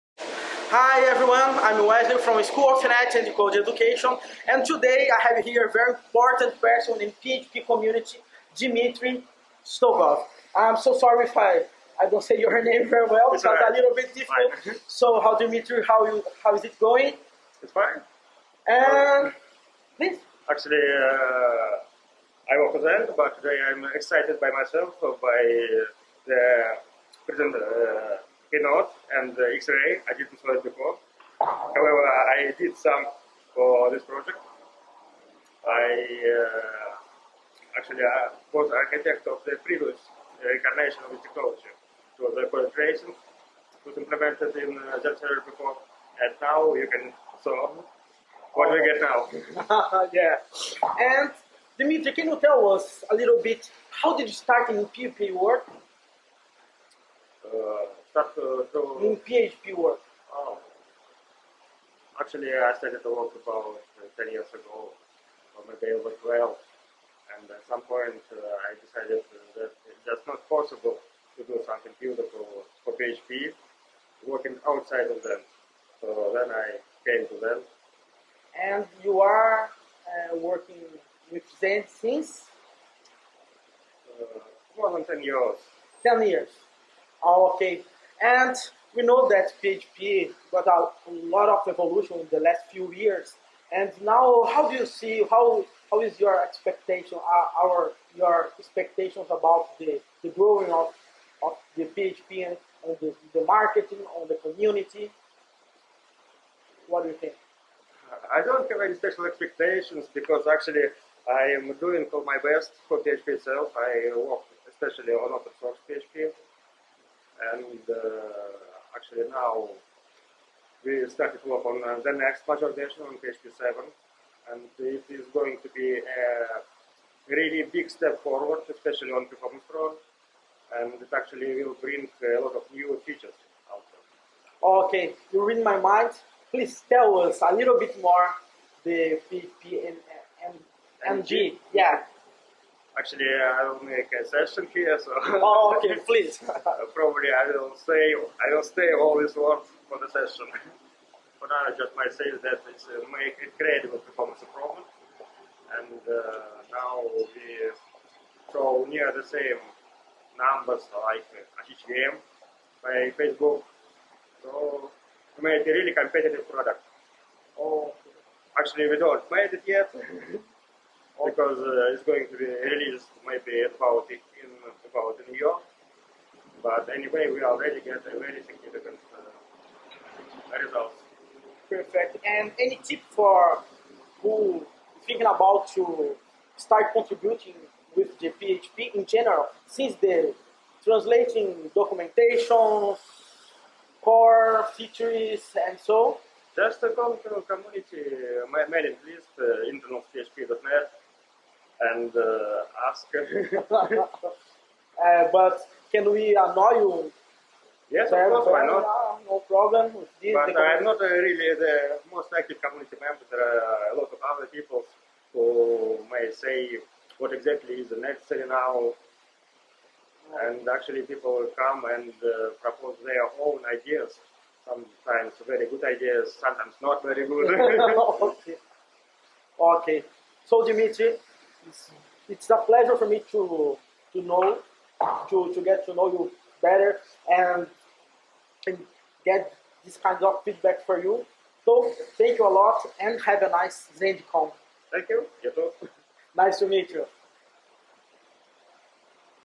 ZendCon 2014: Dmitry Stogov (o cara do PHP-NG)
Entrevista com Dmitry Stogov, um dos responsáveis pela criação do PHP-NG.